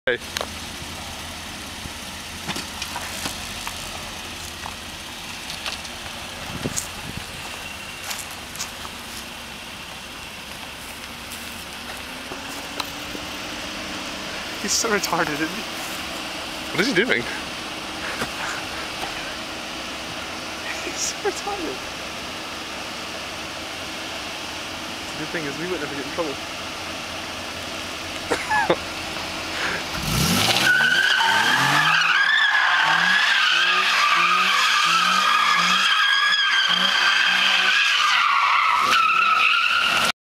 2009 Infiniti G37 Donuts
Getting rid of that annoying excess tread my tires have.